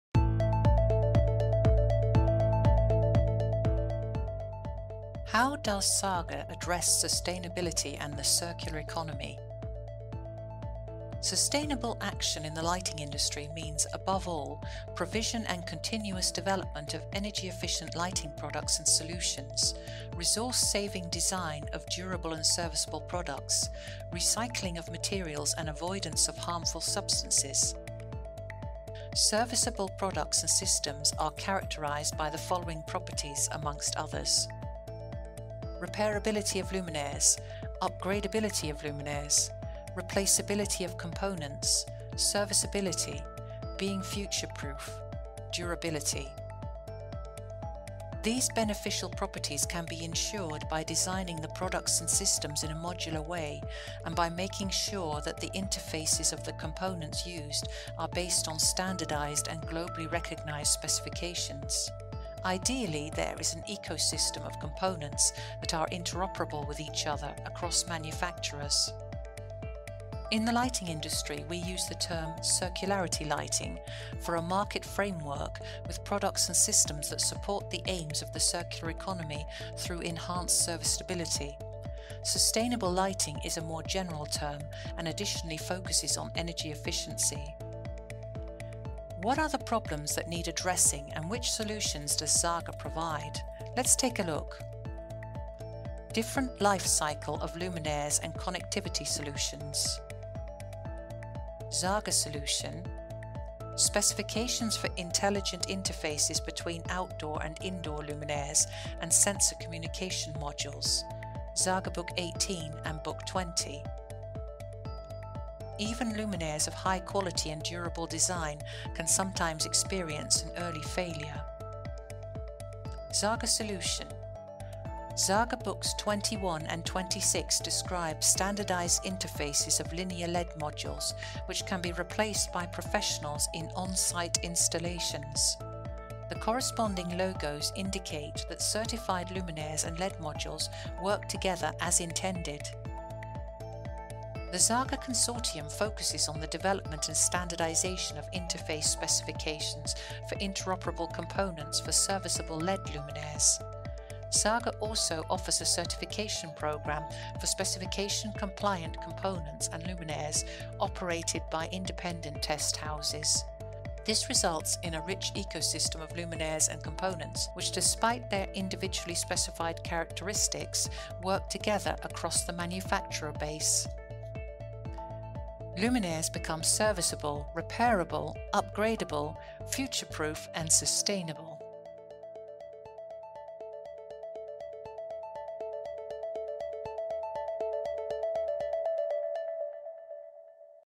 Circularity Lighting at a glance
Durable, repairable and upgradeable LED luminaires are key elements contributing to sustainable lighting. Zhaga is developing and standardizing interface specifications for components of serviceable luminaires, to help facilitate a new market framework called “Circularity Lighting”.